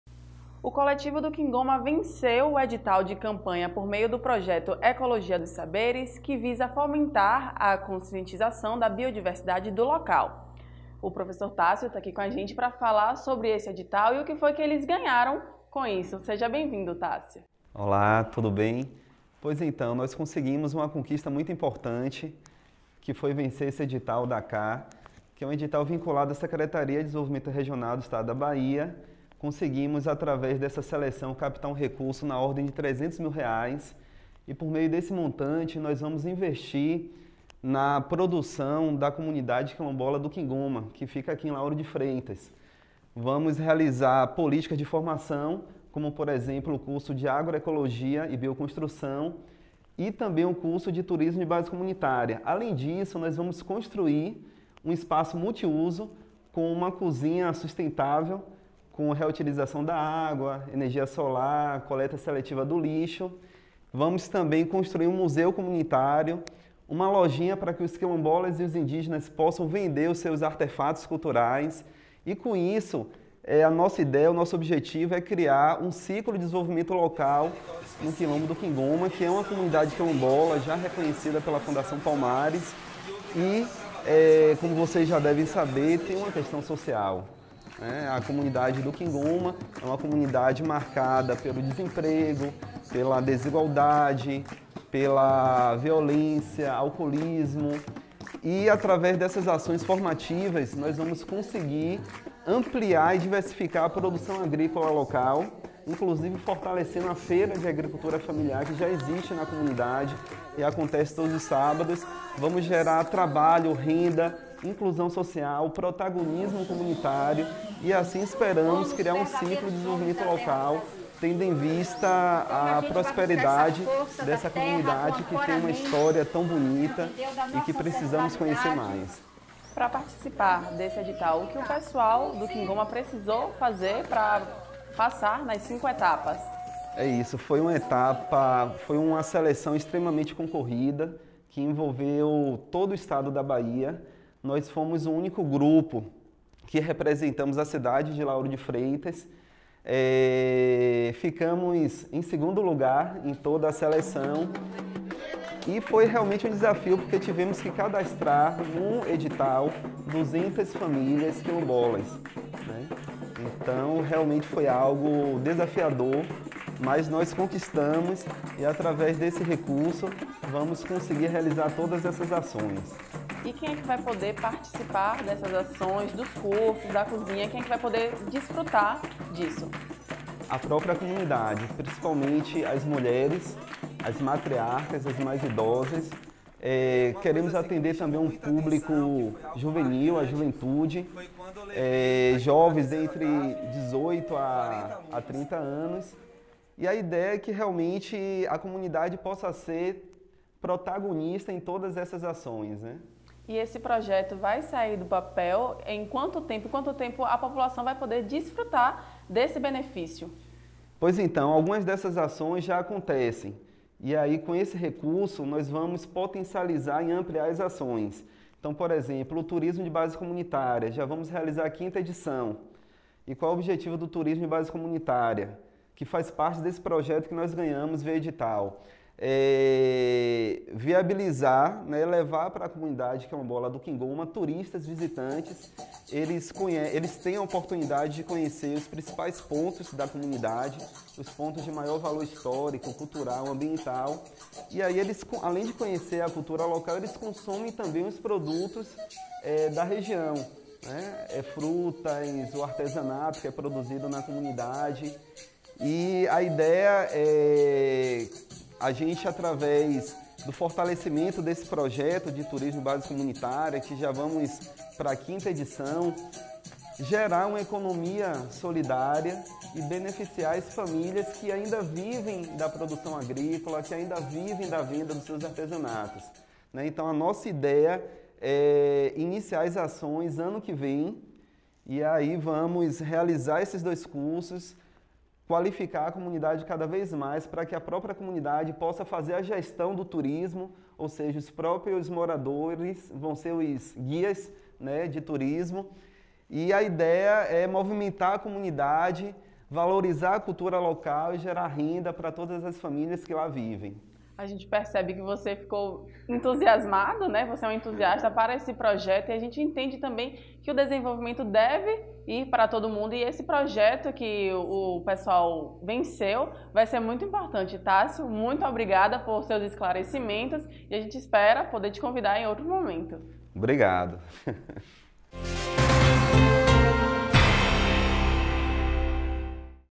Quingoma ganha Projeto Ecológico
Por meio do projeto ecologia dos saberes, o coletivo do Quingoma vai receber 300 mil reais é para conservação e uso consciente da biodiversidade do território.